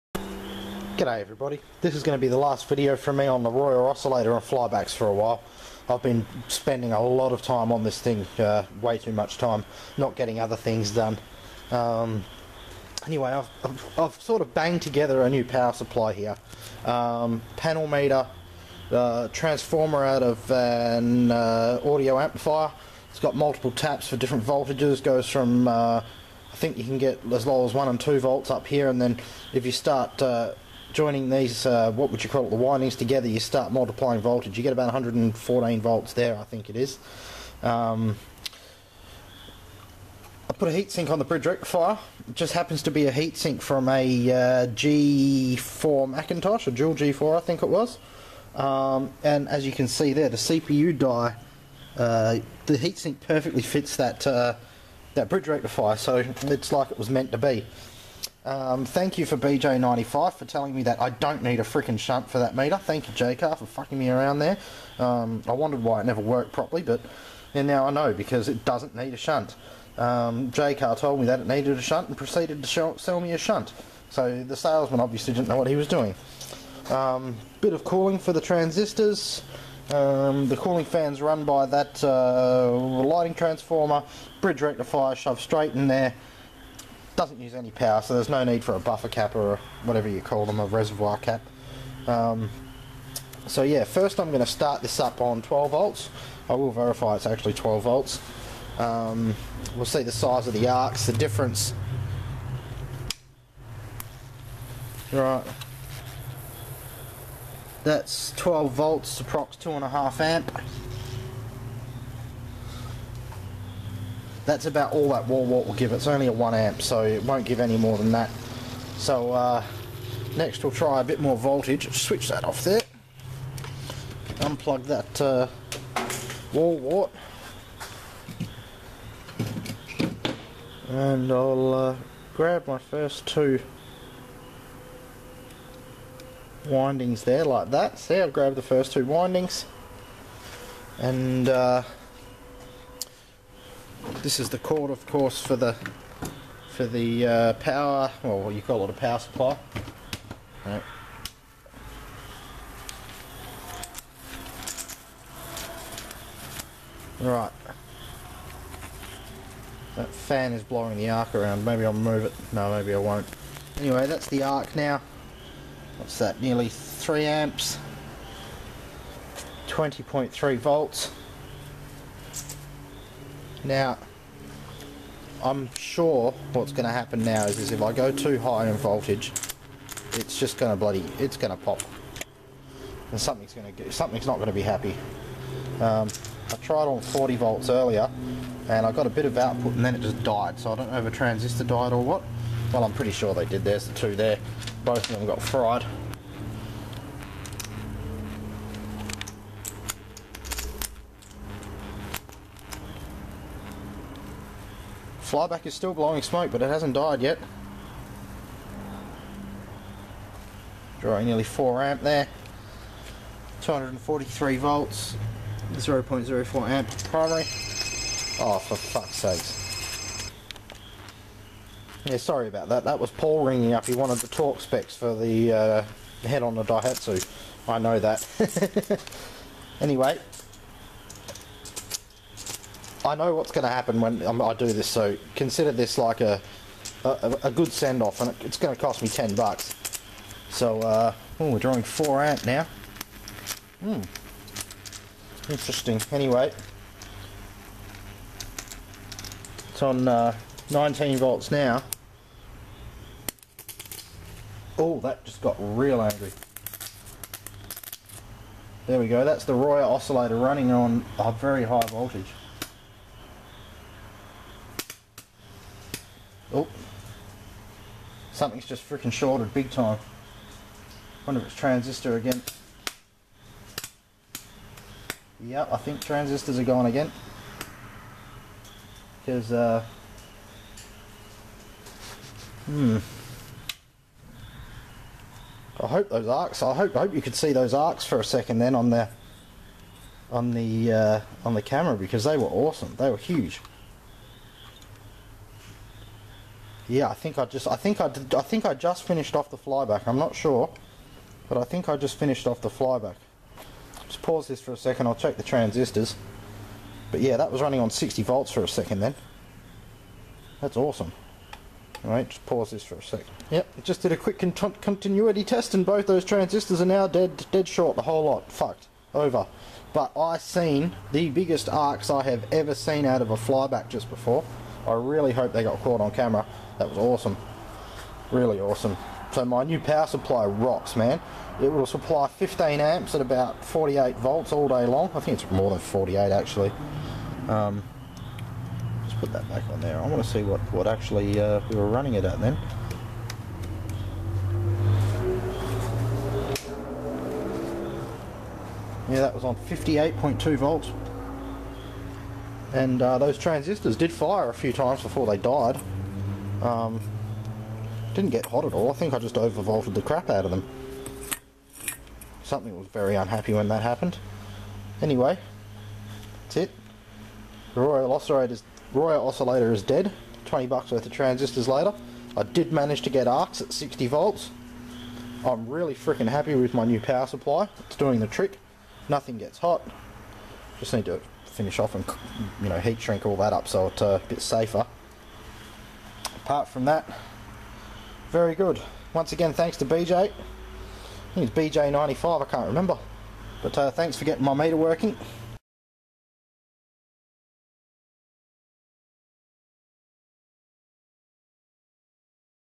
Royer Flyback, Transistor POPs, 12-20-60 Volts & New Power Supply
As title says. Thanks to people credited in video!
nice new home brew power supply with plenty of amps!
you can tell the moment the transistors pop when theres the sharp discharge then nothing LOL
Thanks to Jake aswell!